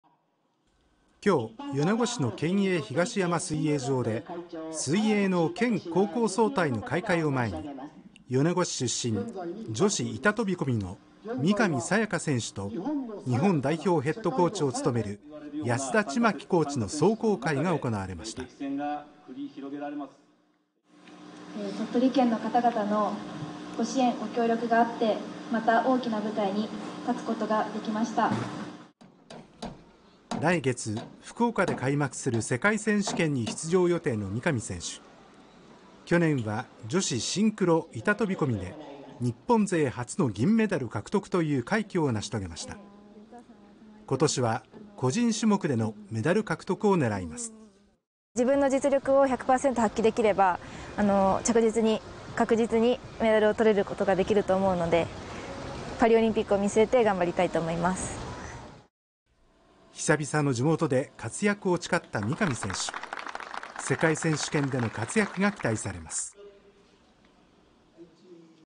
飛び込み・三上紗也可選手 世界水泳に向け地元で壮行会
７月、福岡で行われる世界選手権に出場する、鳥取県米子市出身の水泳・飛び込みの三上紗也可選手。２４日、地元で壮行会が行われ、活躍を誓いました。